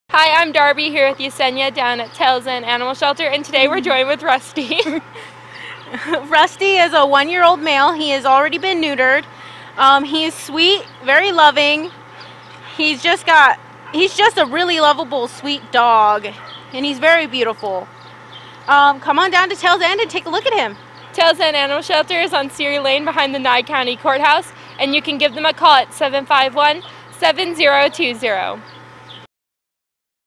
04/03/2015 Save-A-Pet Tails End Animal Shelter
Darbie & Yesenia bring us this week’s Save-A-Pet